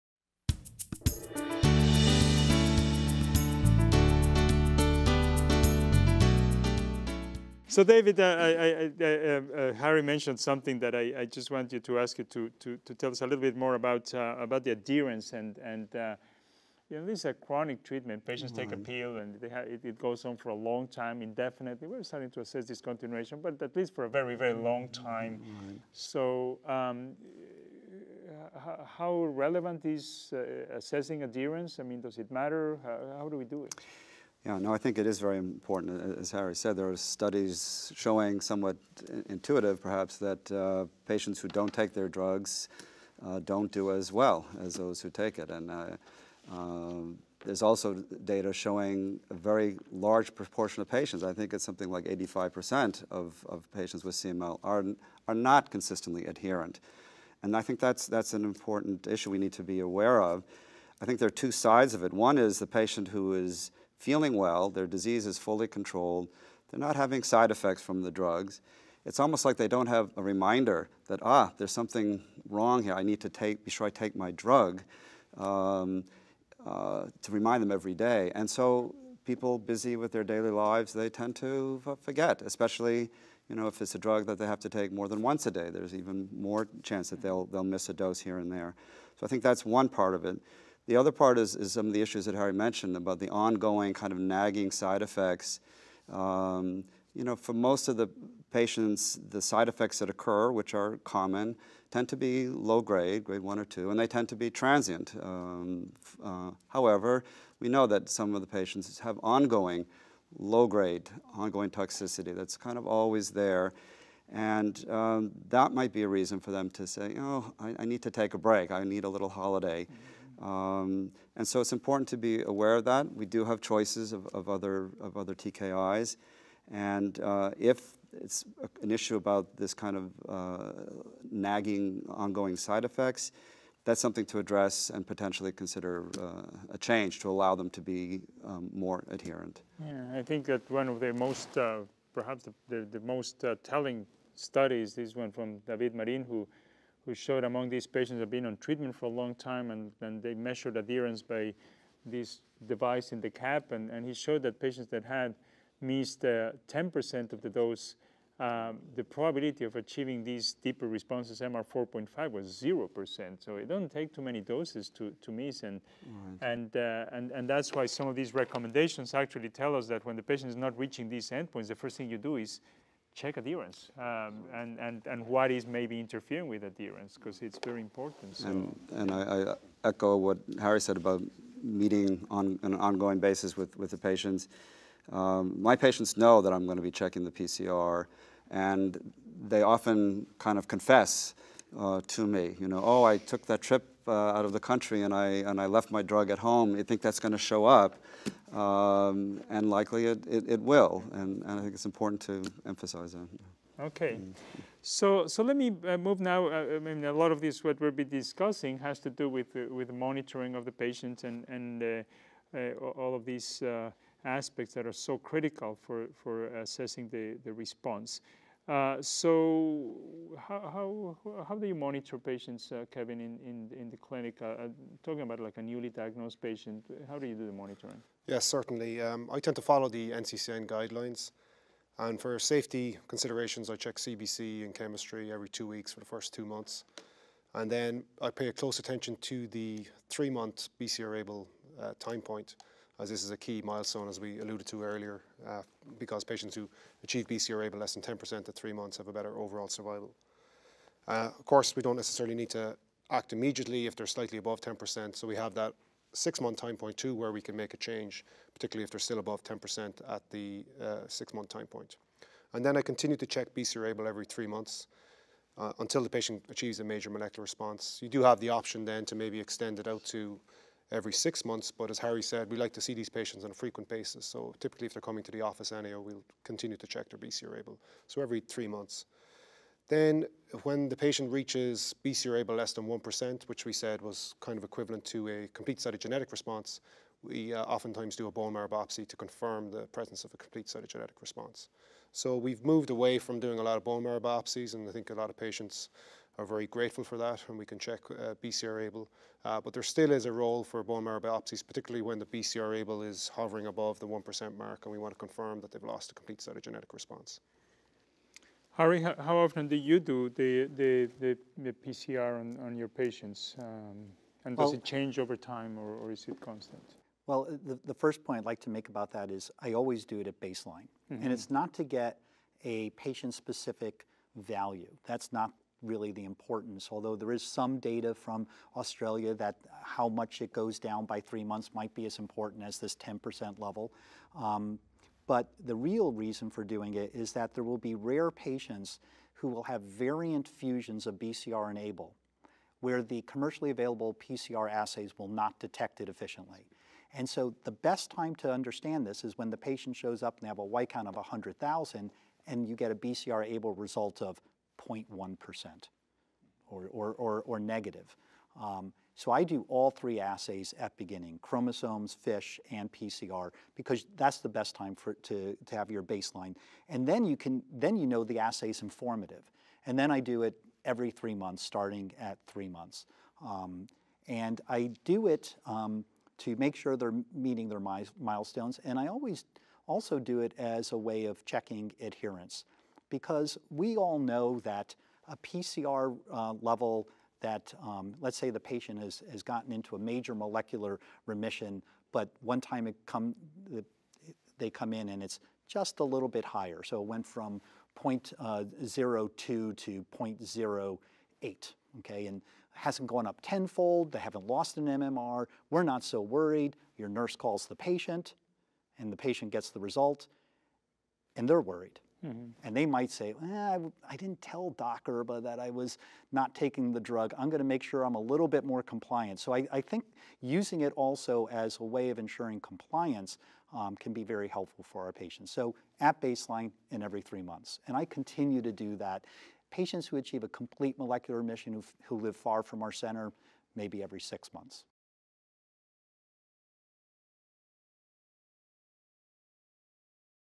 Patient Monitoring in Chronic Myeloid Leukemia (CML)
The panelists, Jorge Cortes, MD; David Snyder, MD, FACP; Kevin Kelly, MD, PhD; and Harry Erba, MD, PhD, emphasize the importance of medication adherence and monitoring patients during treatment for chronic myeloid leukemia (CML).